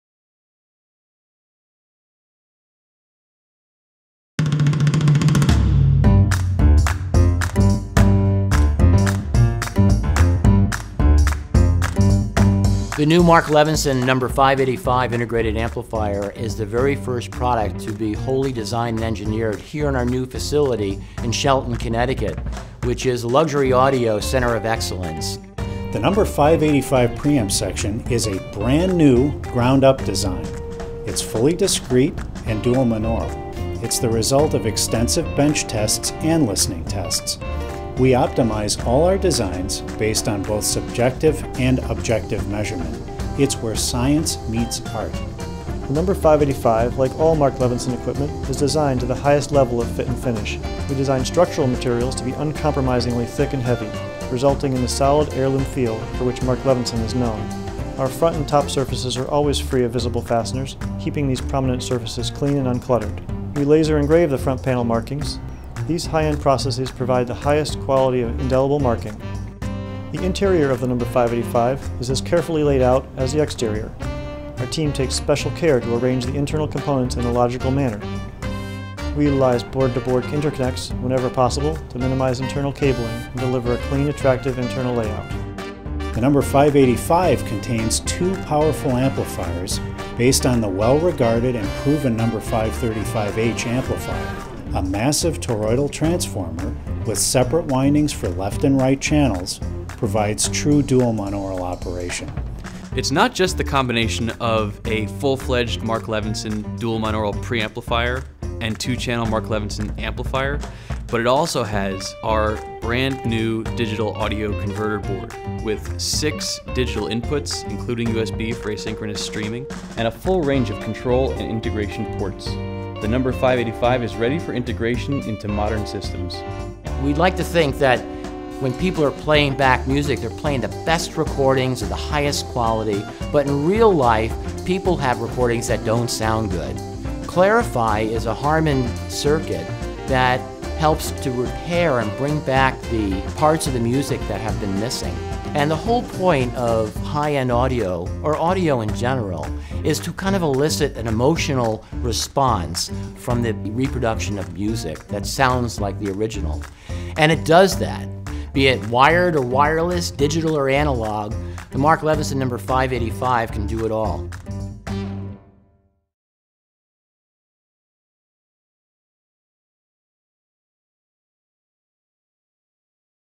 A behind the scenes look at the Engineering Center of Excellence
Take a behind the scenes look at Mark Levinson's Engineering Center of Excellence. Our new No585 integrated amplifier is the very first product to be fully designed and engineered in our new facility in Shelton, Connecticut.